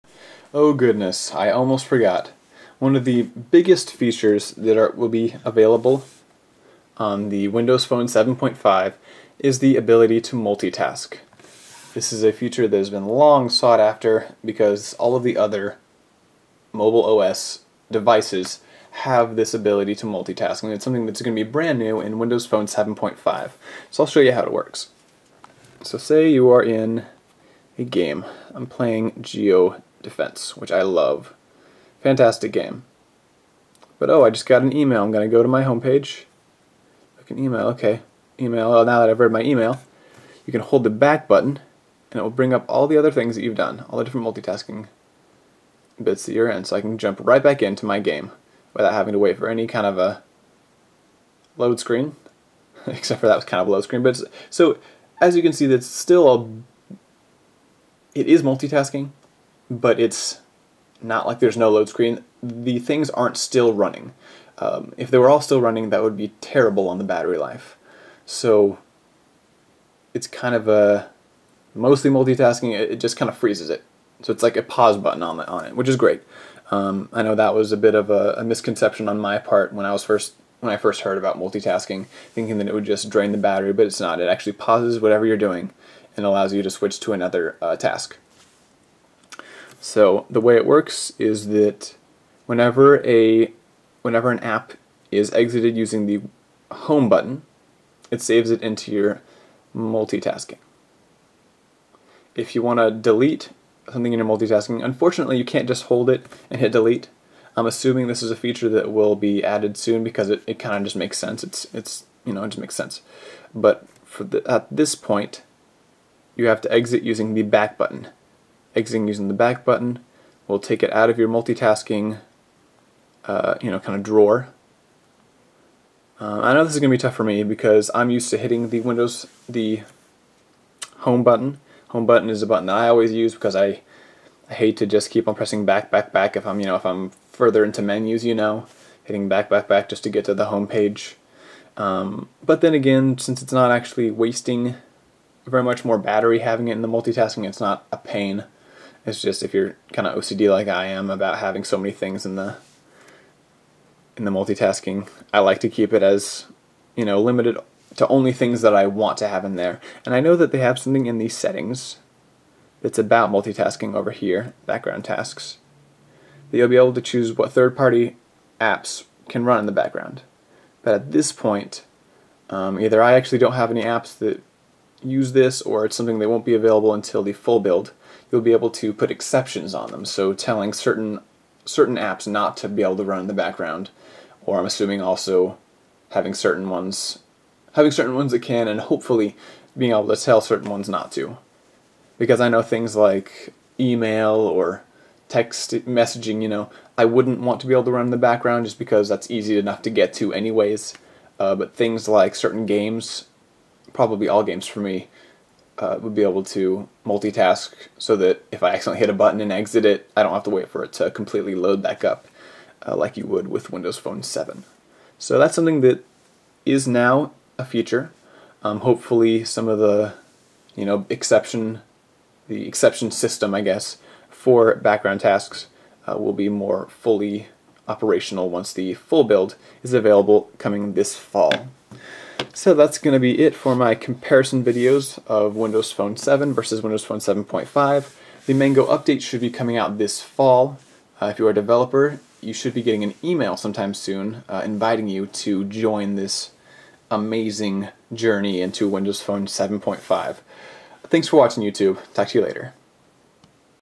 Windows Phone 7 vs 7.5 - PART V: Multitasking
In this series of videos, I will be comparing Windows Phone 7 to to the updated 7.5 Mango version.

In this segment, I talk about perhaps the most anticipated feature coming to Windows Phone via the Mango update: Multitasking.